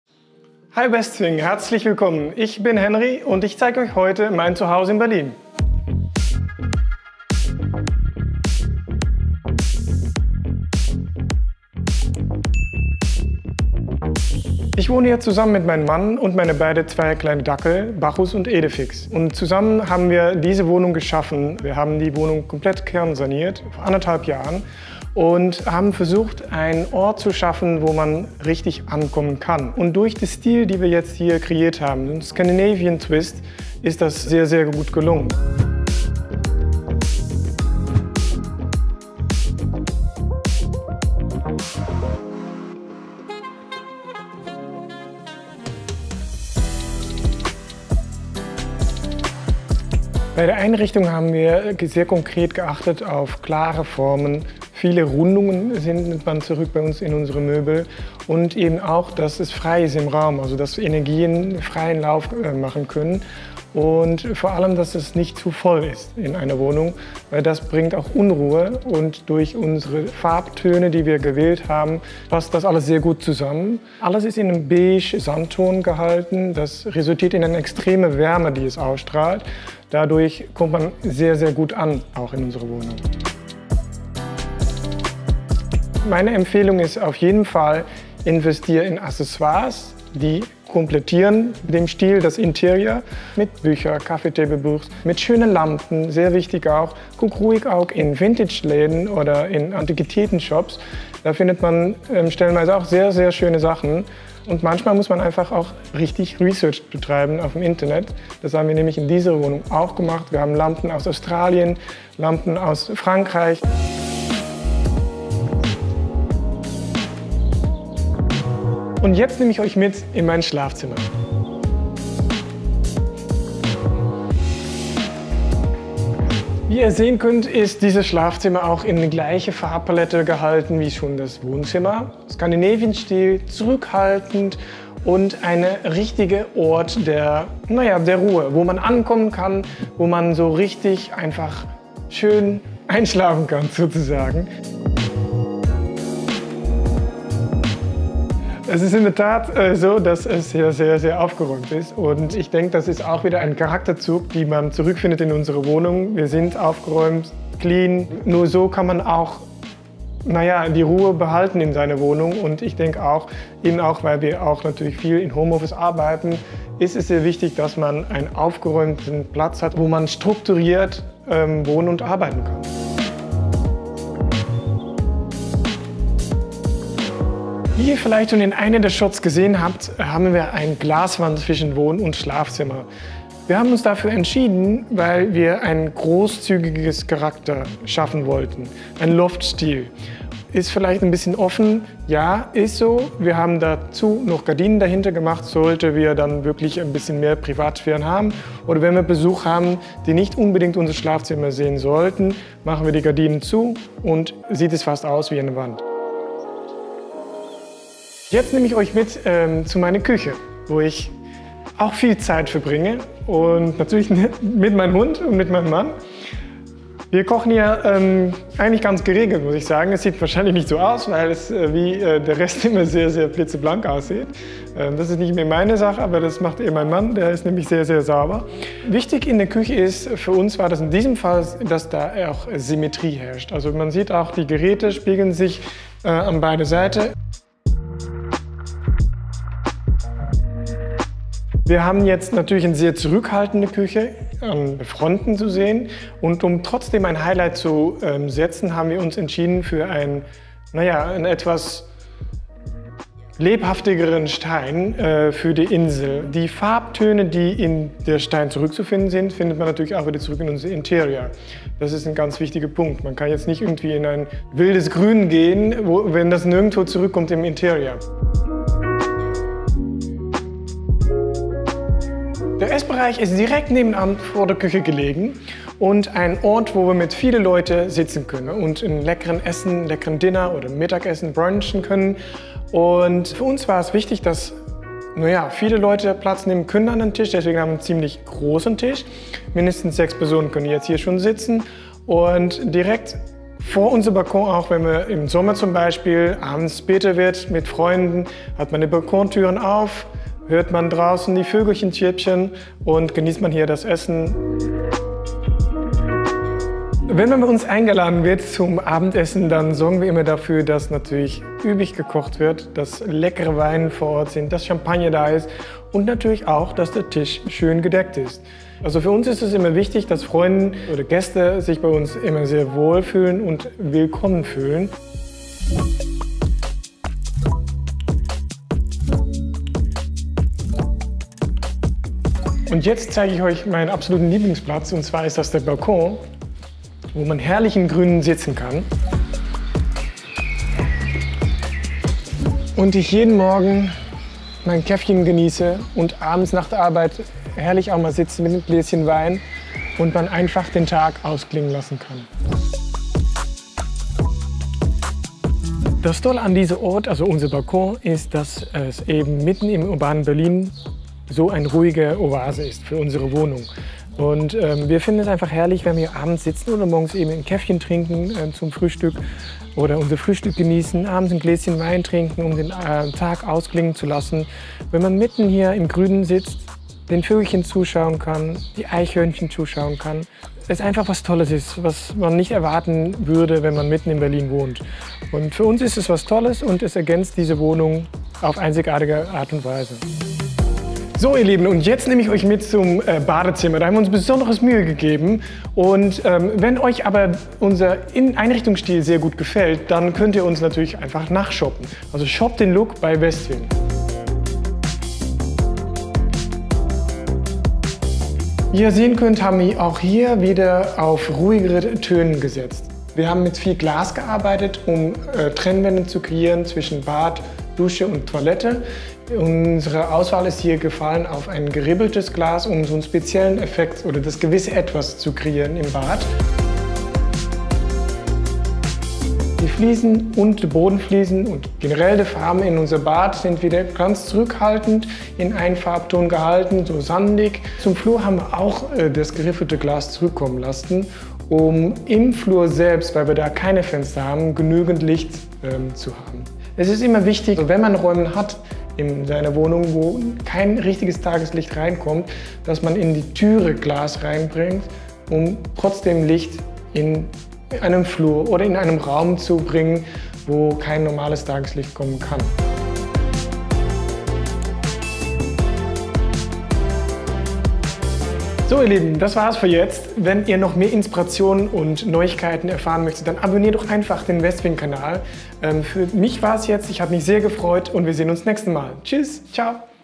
Roomtour | Luxuriös wohnen | Elegante Einrichtung mit modernem Touch
Luxuriös wohnen – wir sind zu Gast bei Hendrikus in Berlin und bekommen ein paar Einblicke in seine Luxus Wohnung. Hier wohnt er zusammen mit seinem Mann und ihren beiden Hunden. Seid gespannt auf Hendrikus elegant Einrichtung mit modernem Touch.

Zu Beginn erzählt Henri, dass sie die Wohnung kernsaniert haben. Durch ihren eigenen Stil – dem „Skandinavien Twist“, wie Henry ihn beschreibt – haben sie das Gefühl bekommen, angekommen zu sein. Das Wohnzimmer hat für das Paar keine allzu große Bedeutung, dennoch haben sie hier auf ruhige, helle Töne gesetzt und wir erhalten erste Eindrücke, wie man ein Wohnzimmer elegant einrichten kann. Henrys Tipp: Setzt auf hochwertige, elegante Deko und Accessoires.

Weiter geht es im Schlafzimmer. Auch dieses wurde sehr elegant eingerichtet und es finden sich die gleichen Farbnuancen, wie auch im eleganten Wohnzimmer wieder. Das Schlafzimmer ist ein Ort der Ruhe. Deshalb hat Henry bei der Einrichtung auch auf ruhige und schlichte Luxus Möbel gesetzt. Wenn man elegant wohnen möchte, ist es zudem wichtig, alles sehr ordentlich zu halten.

Zum luxuriösen Wohnen gehört natürlich auch eine Luxus Küche. Hier hat das Paar auf ruhige Fronten und Symmetrie gesetzt. Das Highlight der Marmorküche ist die großartige Arbeitsplatte aus Marmor. Zuletzt bekommen Wir noch Einblicke in das wunderschöne Luxus Bad des Paares. Das Badezimmer ist sehr hell und passt ideal zur restlichen glamourösen Einrichtung. Das Highlight hier: die freistehende Badewanne. Ein Traum!

Luxuriös wohnen! So schön kann elegante Einrichtung mit modernem Touch sein. Wie hat euch die Luxus Roomtour gefallen?

Videoinhalt:
0:00 Intro
0:36 Einrichtungstipps
0:47 Wohnzimmer
1:58 Schlafzimmer
3:28 Küche
4:32 Essbereich
5:32 Balkon
6:46 Bad